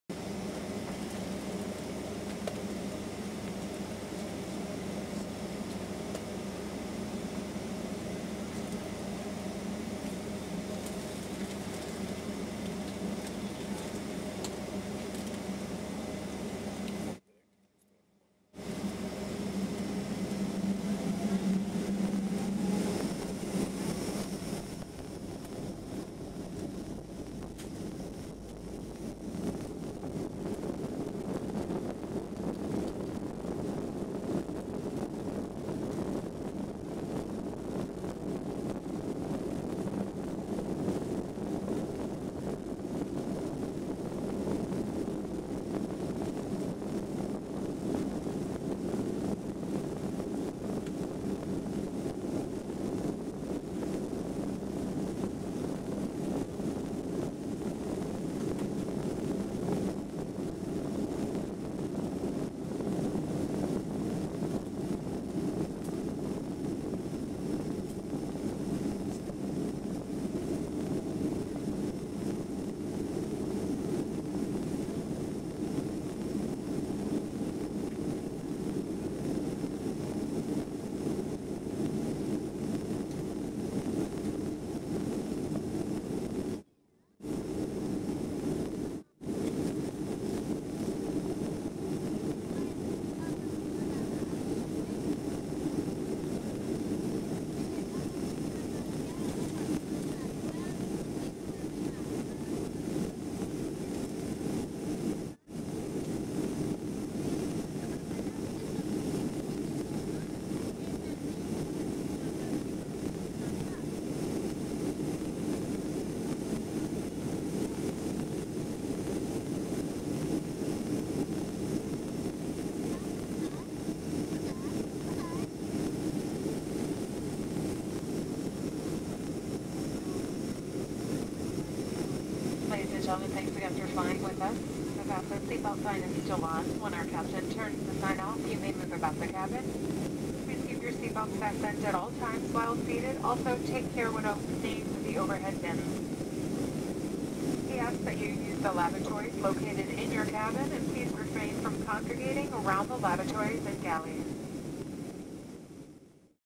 United Boeing 757 - Take-Off from Chicago O'Hare
United Boeing 757, registration N580UA, flight UA681, taking off from Chicago O'Hare the 7th of June 2014 heading for San Francisco - SFO

I recorded and uploaded this for fun and it's not supposed to be on any sort of professional level.